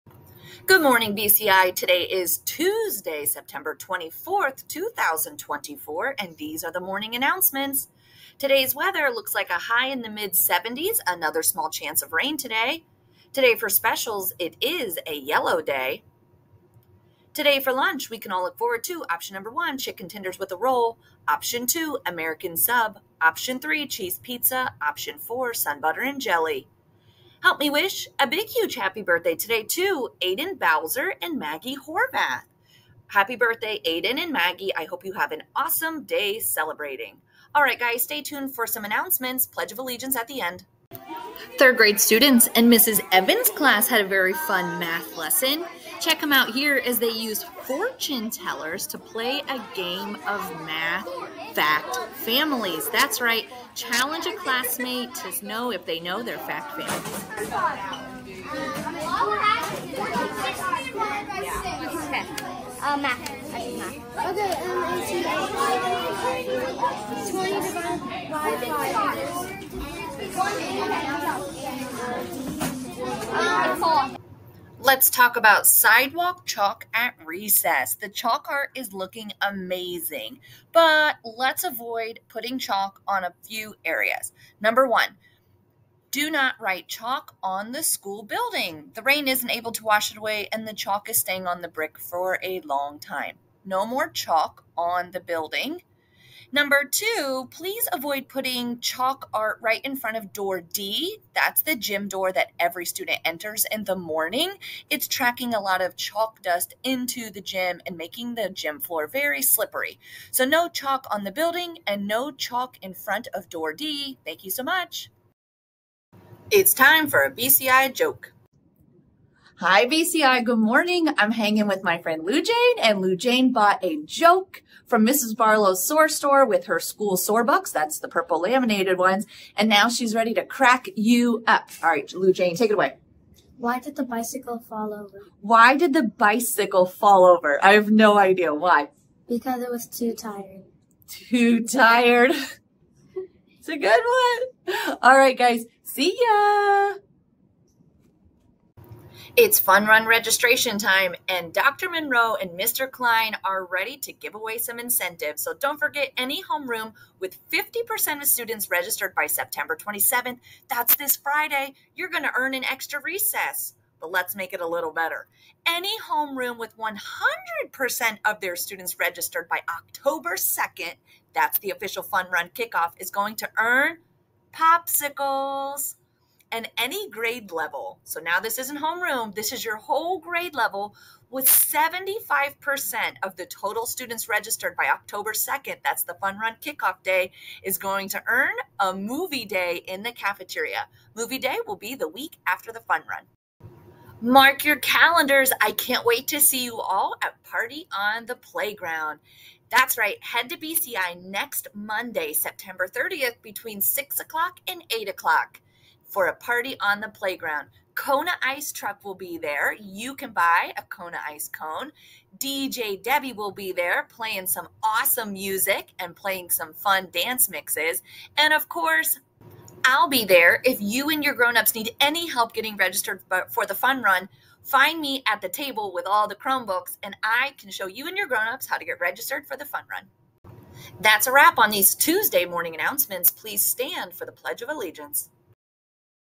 Tuesday 9 24 24
Chalk art rules, Fun Run Registration, Party at the Playground, Mrs. Evans math game, Lujain joke